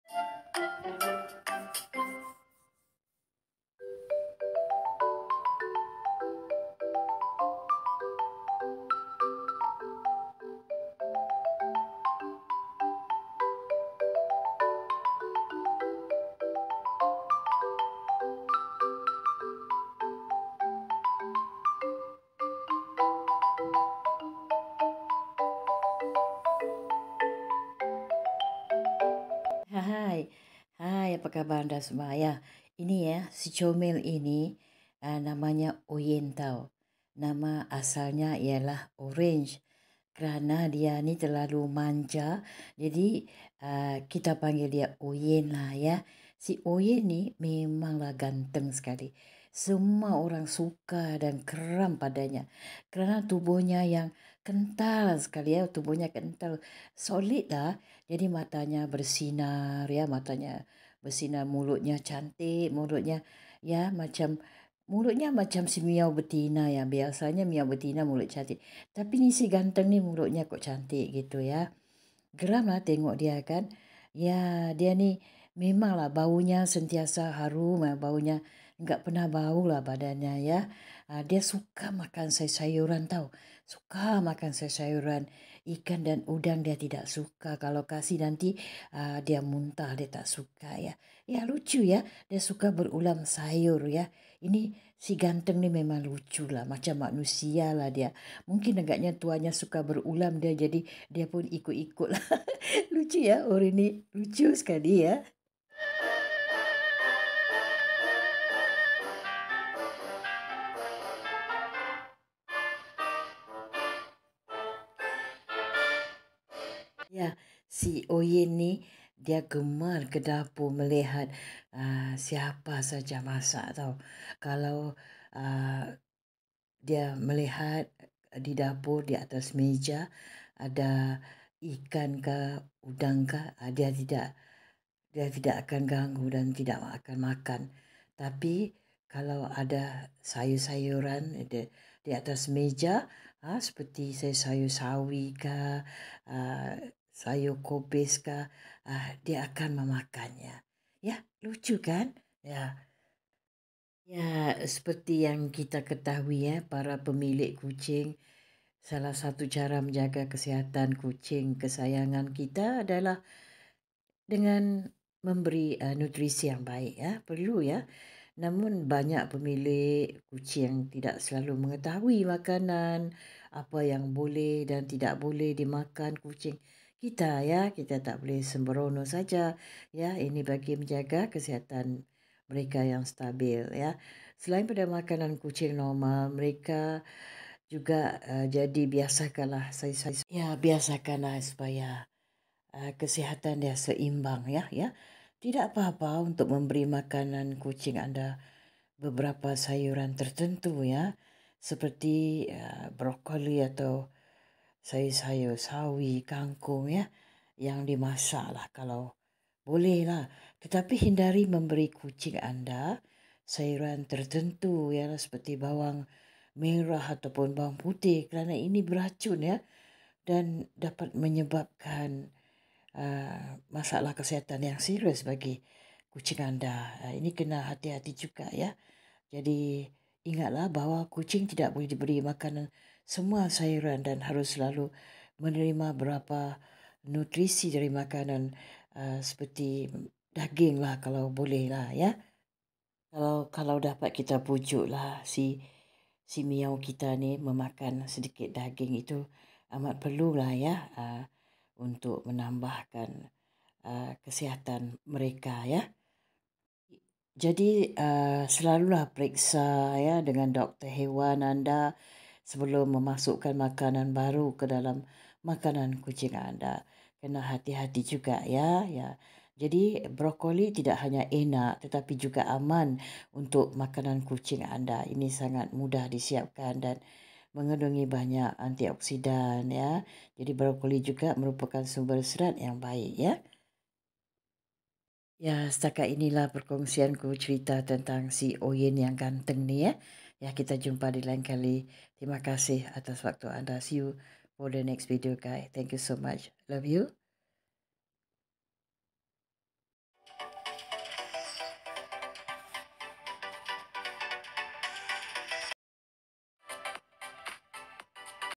Nik Fatima"Oyen Ganteng"# cat lovers#cute cat#
cats lovers# cute cat#
kucing jangan ini namanya Oyen,
memang comel, Bukan saja comel
malahan lucu
suka makan sayuran, ikan Dan udang
tak makan, tercium bagu udang Aja muntah
lucu ya Kan, seperti perempuan Yang sedang
mengandong, oyen suka bermain Tali
Dan Bunga mangga
selamat menonton